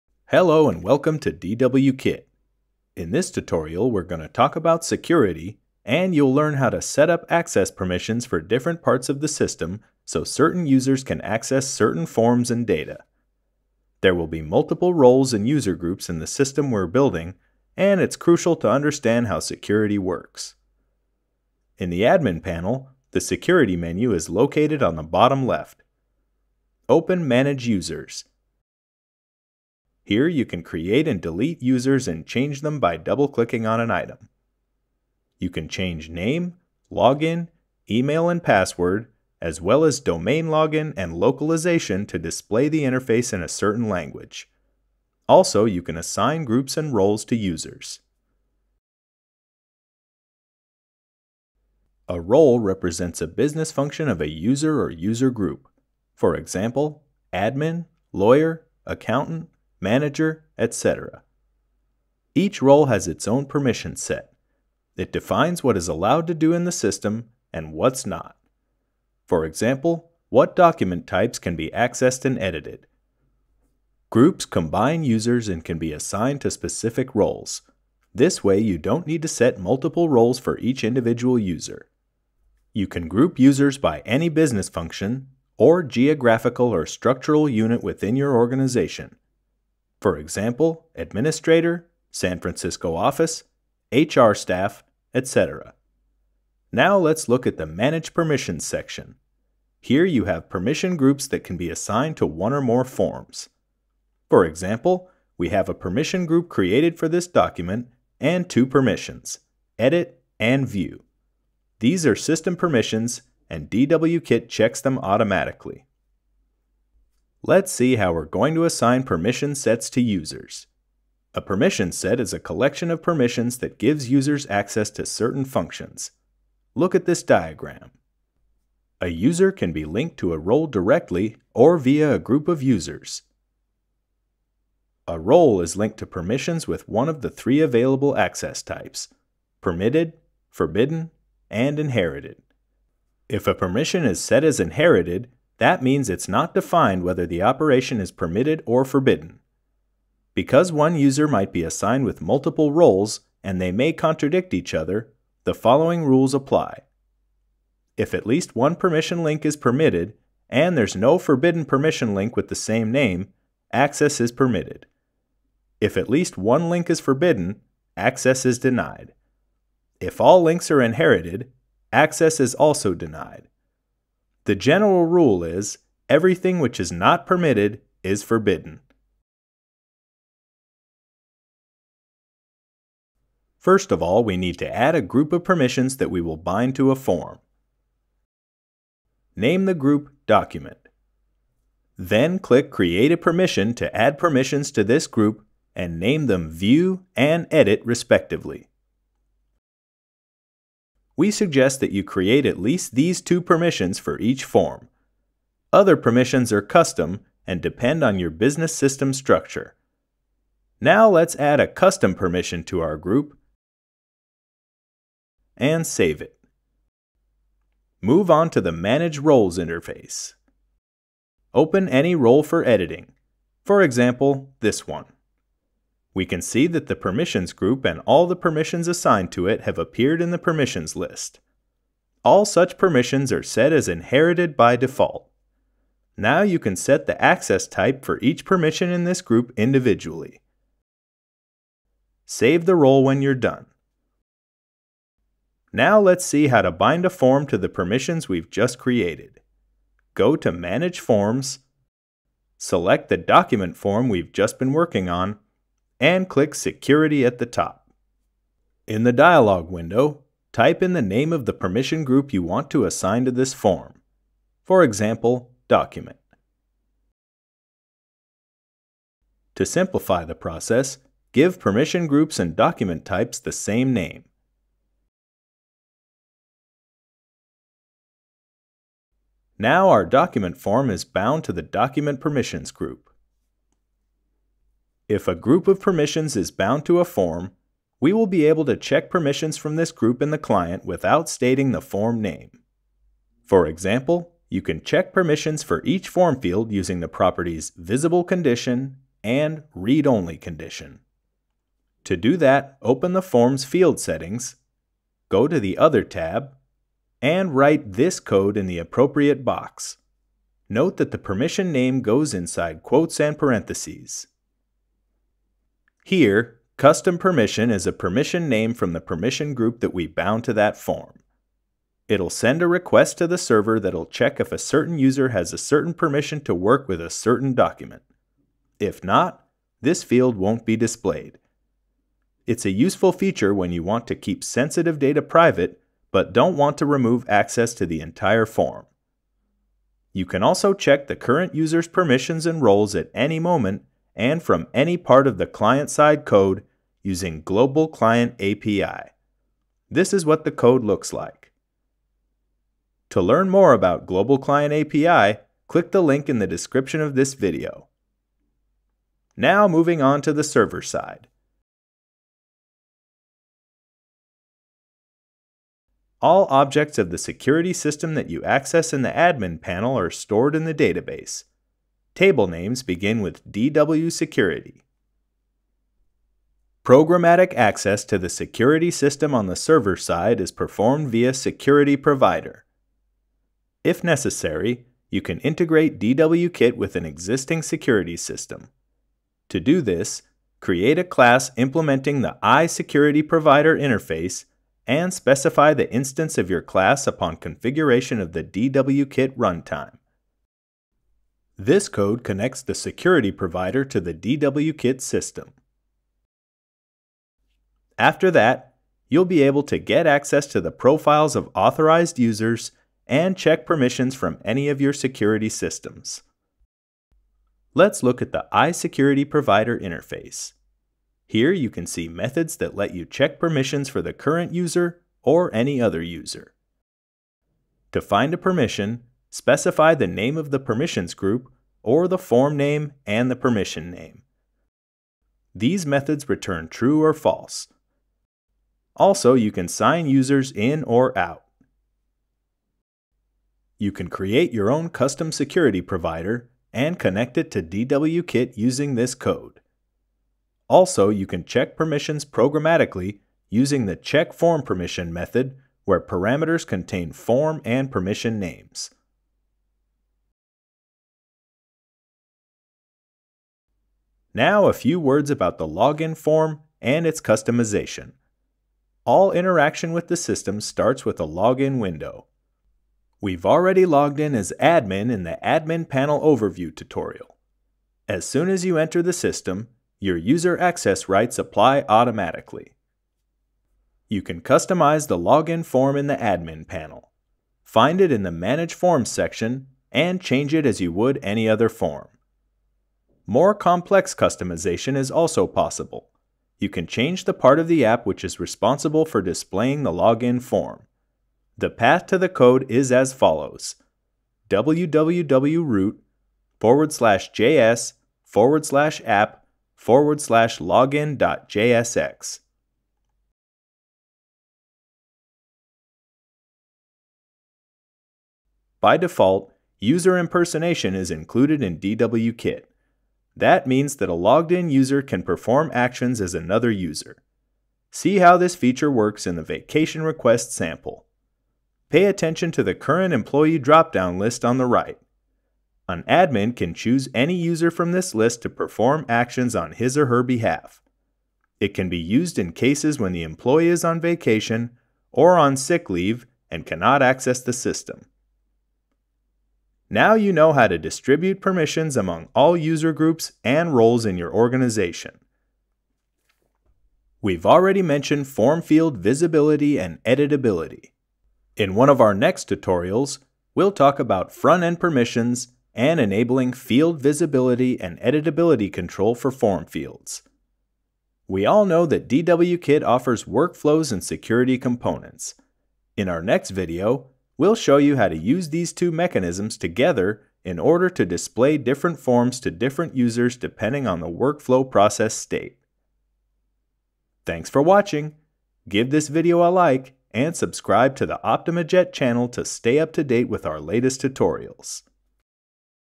Security, Permissions, User Roles and Groups
In this tutorial we are going to talk about security and you will learn how to setup access permissions for different parts of the system, so certain users can access certain forms and data.
Overview 00:14
Binding a form to the permissions 03:00
Permission management on the server 06:03
Login form customization 07:40
User impersonation feature 08:35